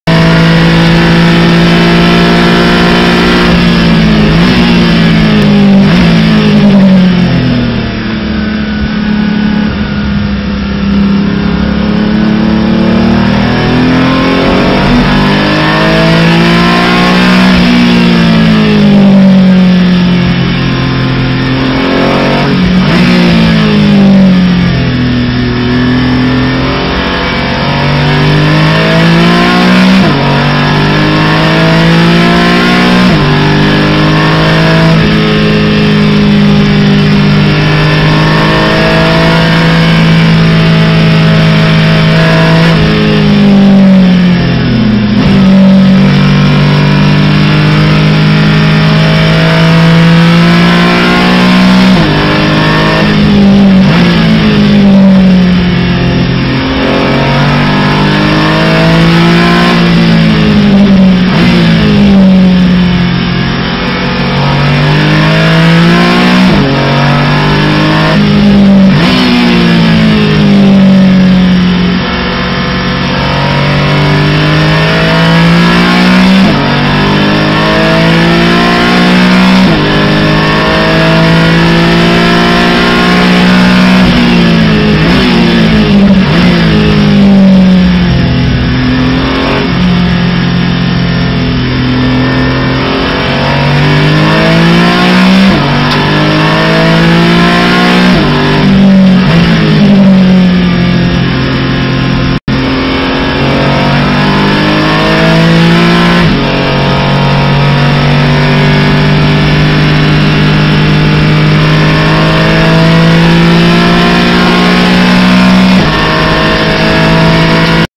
RN #1 Onboard video Zandvoort, Default Vehicle, 01:56.919
GUNTER VDB in Default Vehicle achieved a lap time 01:56.919 on Zandvoort on the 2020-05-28 at 16:06.